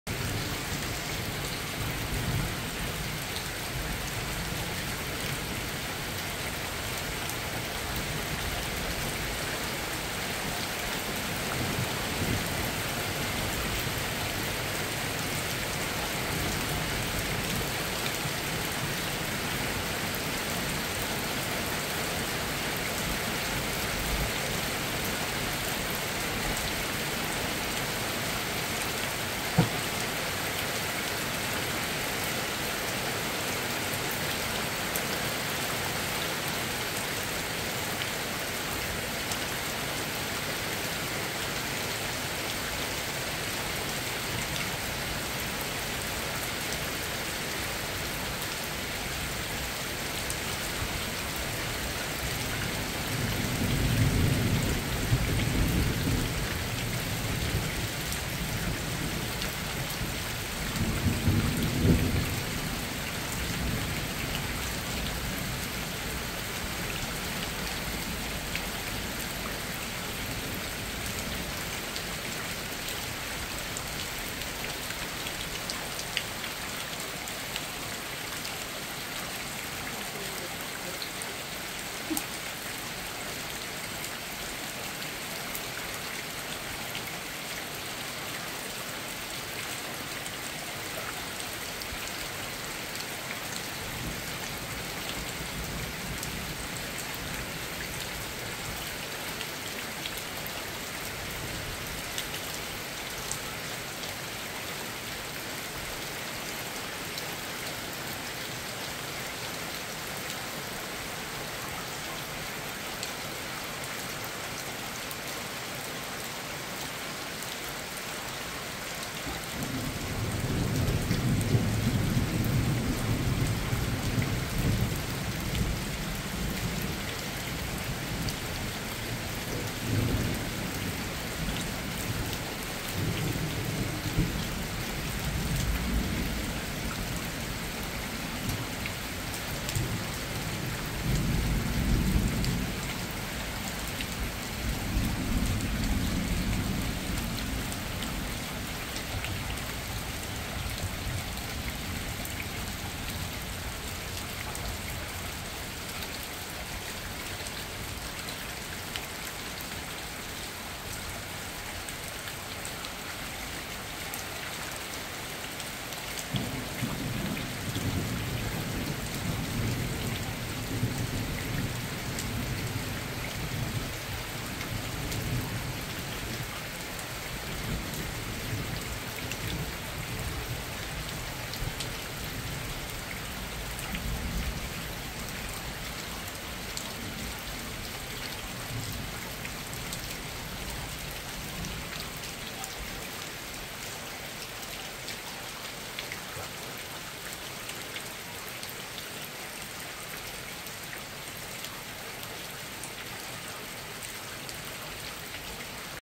아침 7시 우리집ㅡ 시원하쥬
강한비가 예보된 오늘 벌써부터 천둥 ㆍ번개에 요란한 비가 쏟아붇네요